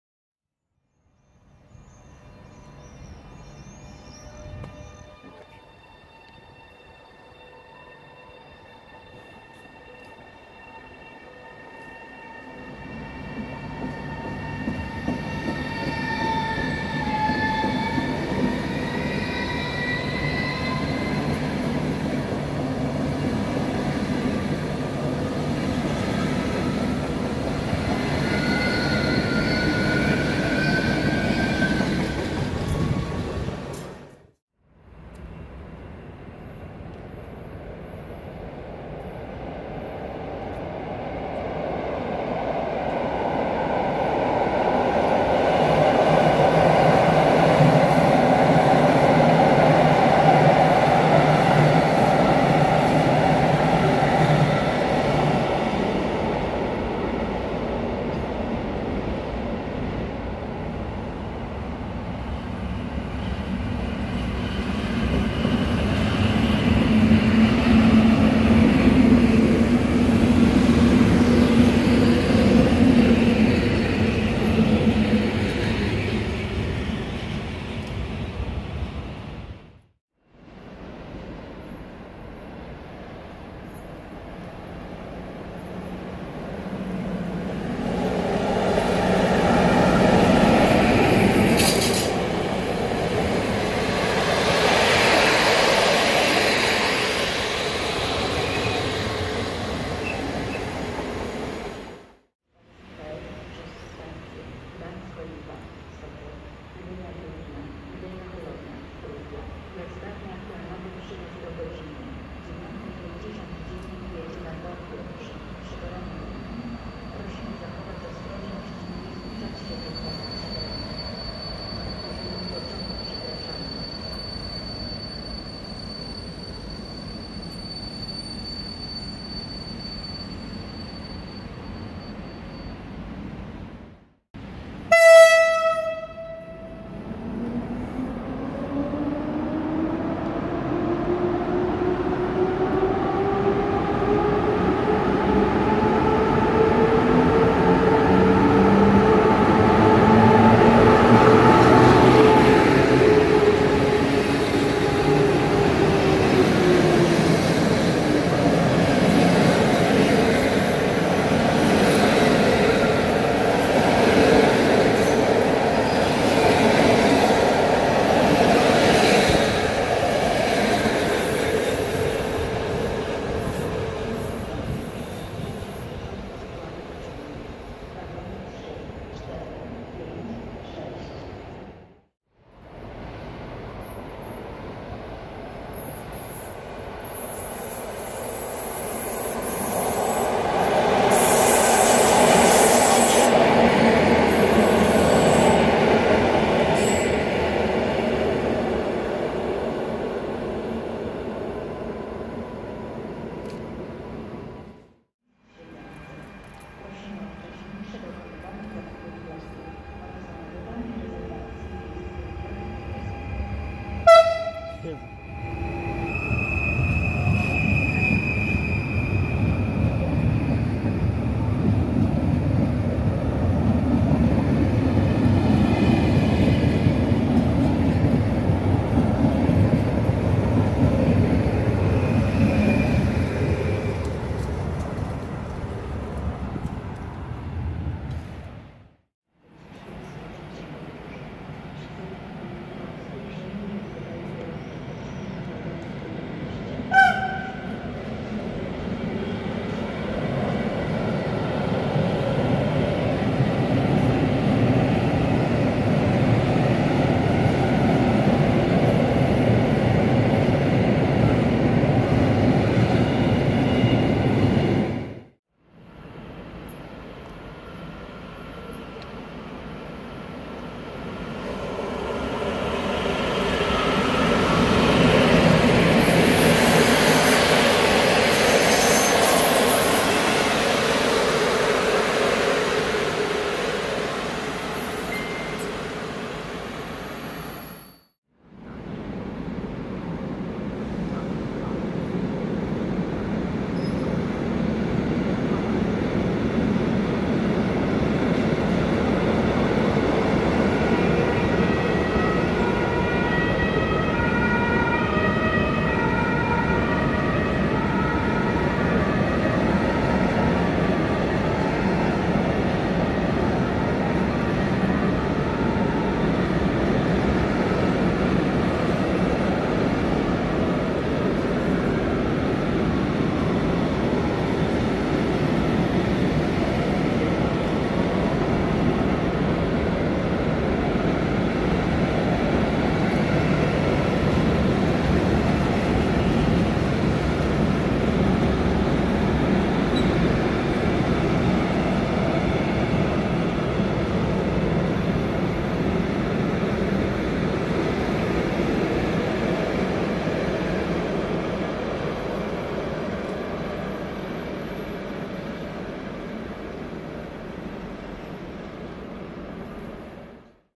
Pociągi w Gdańsku "część 1"
niedługo pojawią się kolejne części :)

1. Godzina 10.39 - ED250-003
2. Godzina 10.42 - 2x EN57AKM
3. Godzina 10.43 - EN71-017
4. Godzina 10.47 - SU42-533
5. Godzina 10.48 - wjazd EP07-456
6. Godzina 10.54 - odjazd EP07-456
7. Godzina 10.57 - EN57ALd-2209
8. Godzina 11.01 - ED160-020
9. Godzina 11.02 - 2x EN57AKM
10. Godzina 11.04 - EP07-1060
11. Godzina 11.13 - EN57
12. Godzina 11.13 - TRAXX PCC E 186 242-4